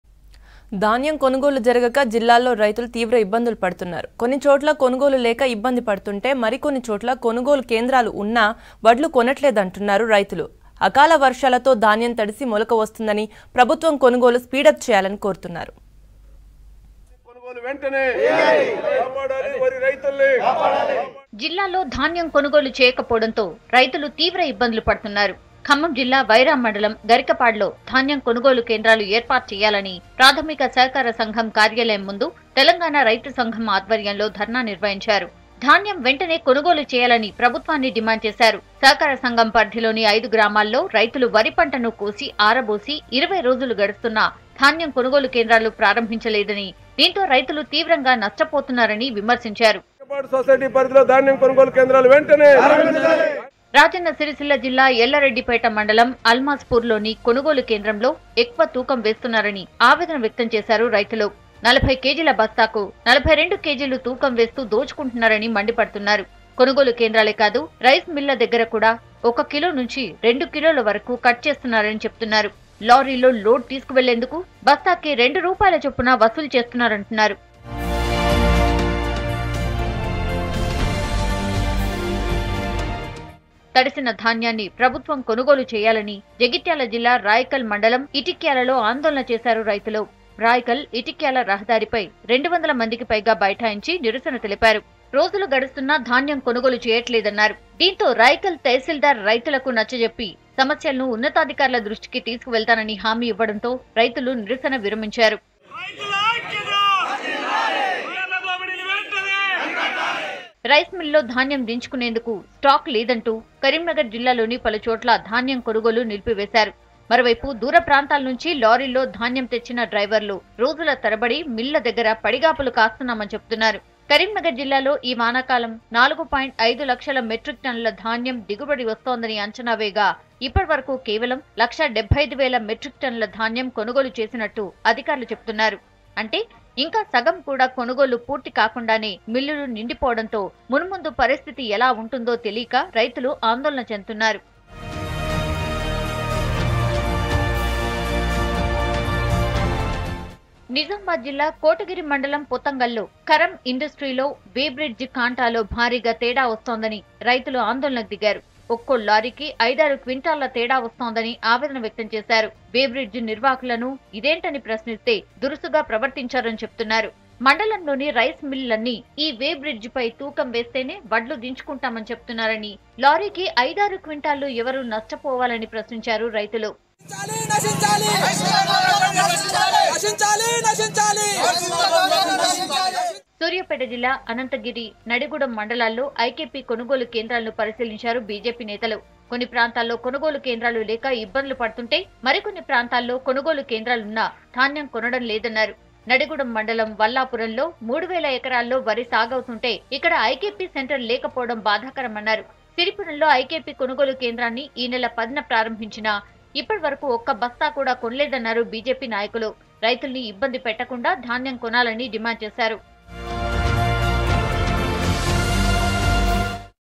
రైతు గోస.. Farmers Protest Over Negligence On Paddy Procurement | V6 News
రైతు గోస.. Farmers Protest Over Negligence On Paddy Procurement